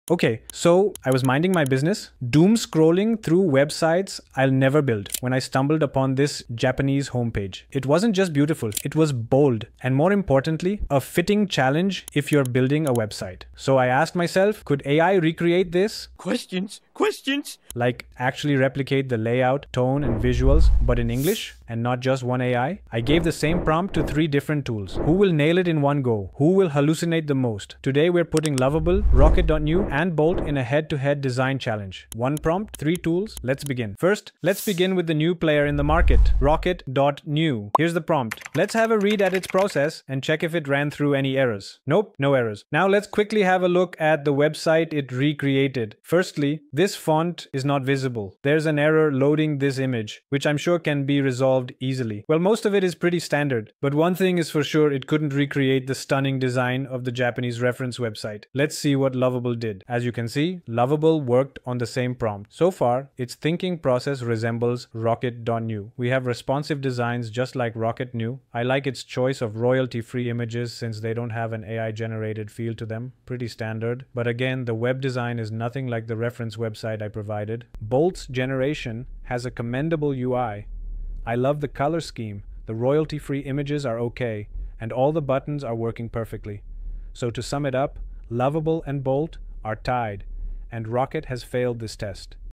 Website Design Challenge: Lovable vs Rocket vs Bolt
I gave one prompt to three AI tools to rebuild a stunning Japanese website — and one of them accidentally made Reddit. You have to see how wild this gets.

Don't forget to:
💬 Share your experiences in the comments below